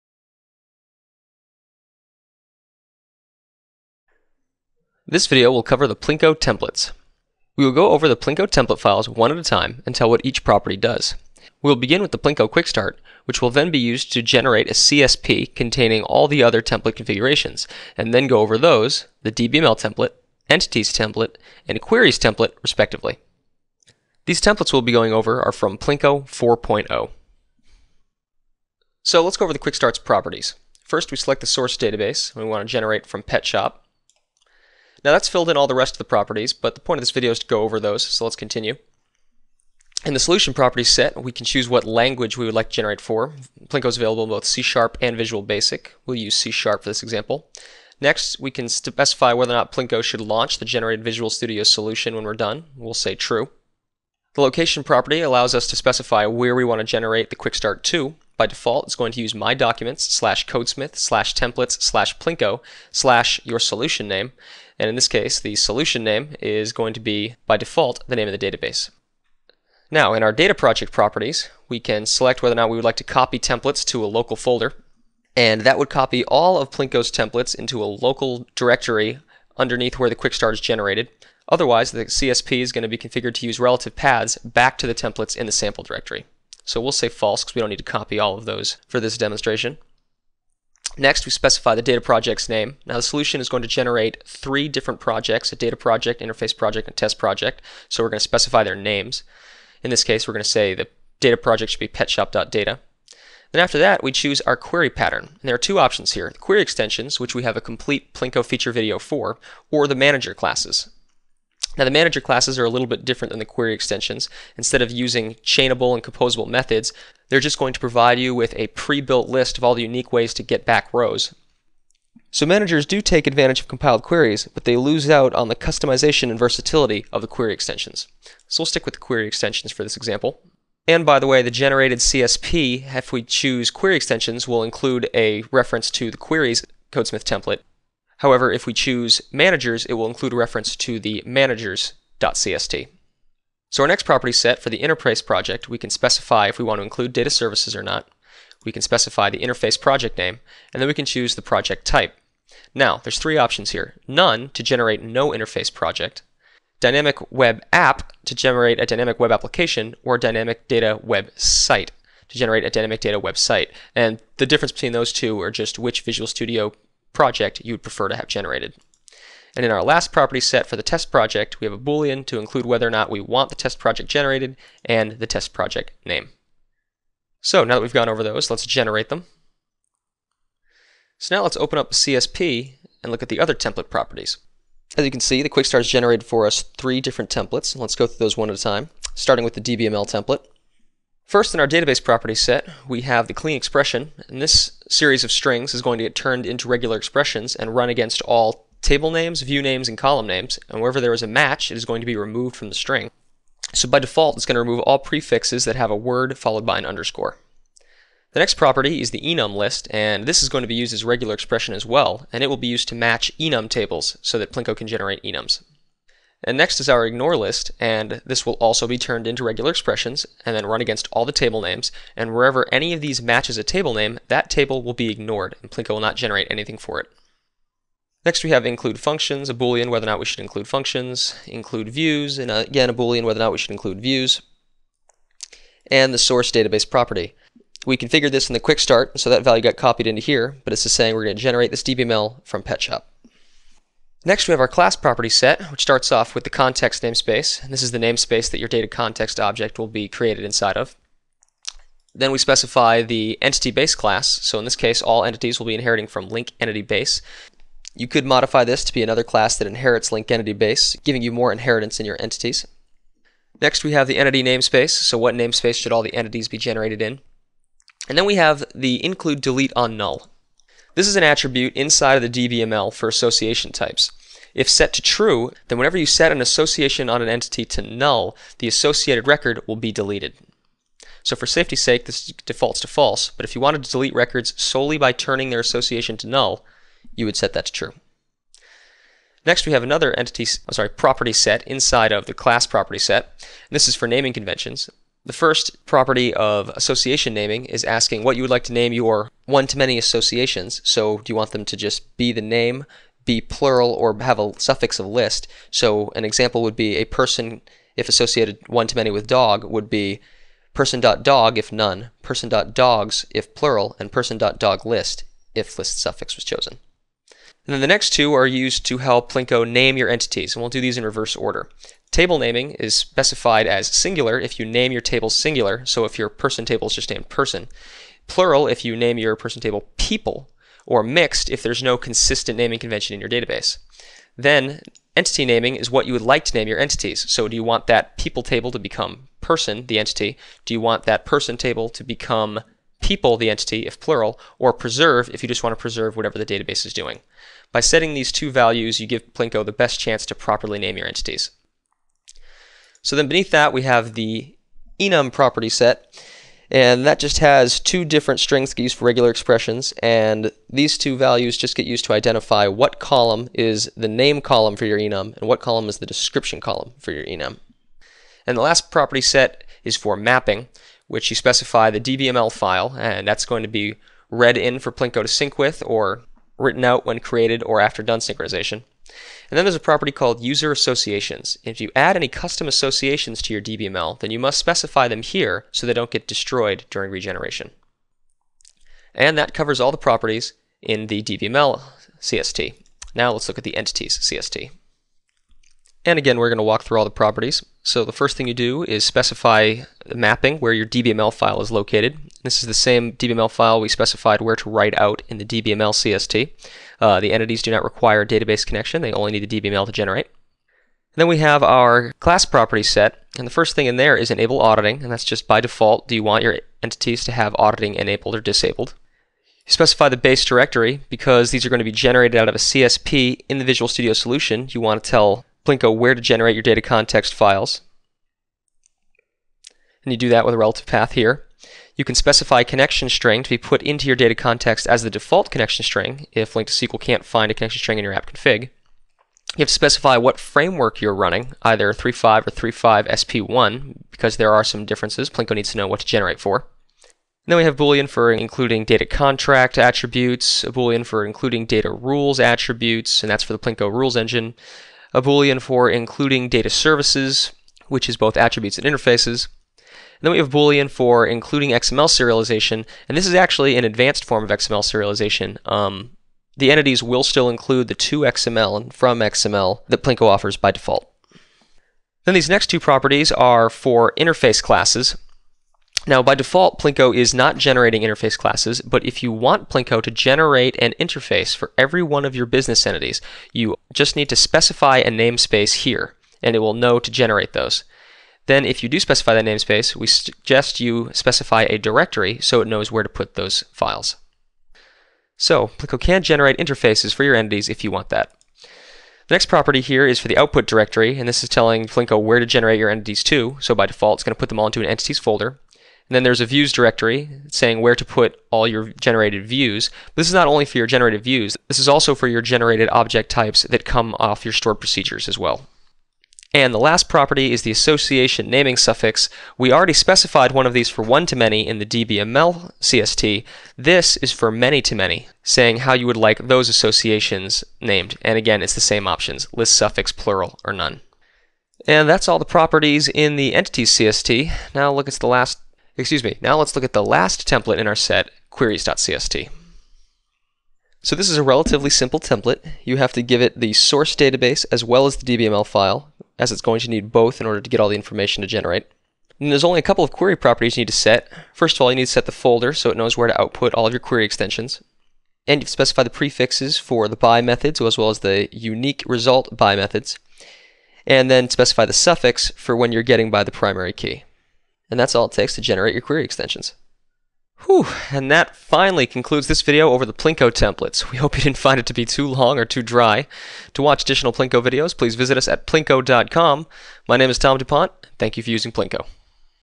PLINQO - Quick Start & Templates
This video goes over the PLINQO quick start and other templates and describes what each template property does.